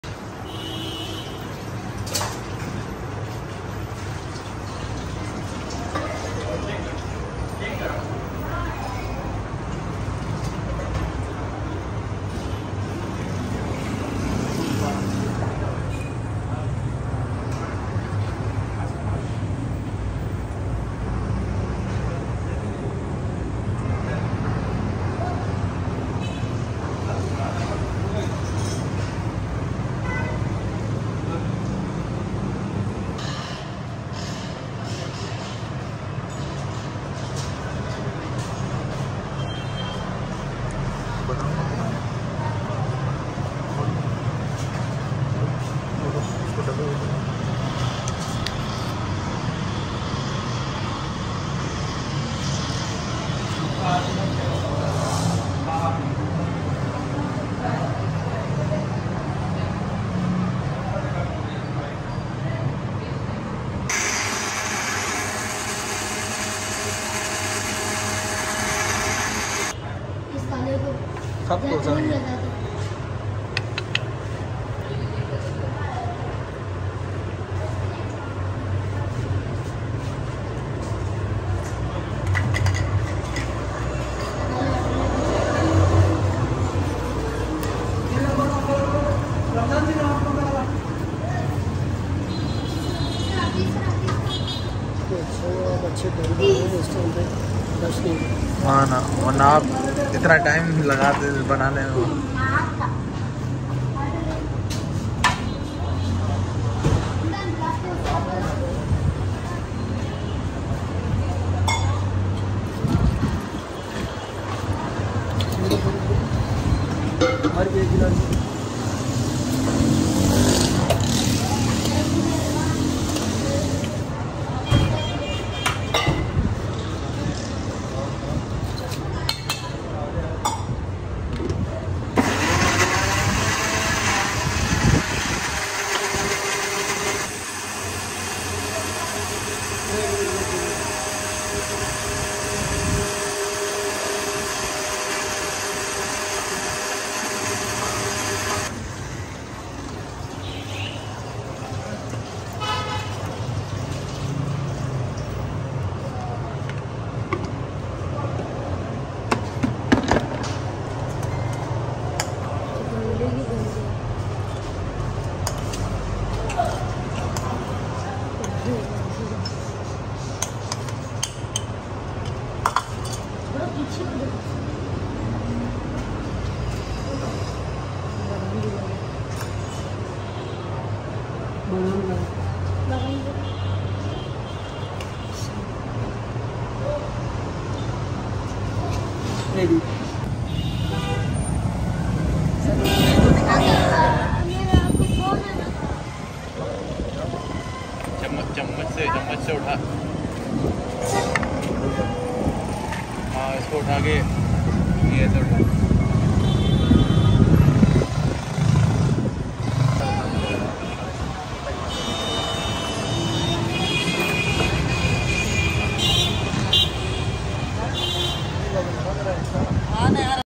How to Make Delicious Mango Shake|Street Food of PK!
Mango Shake Street Food of PK
How to make mango shake.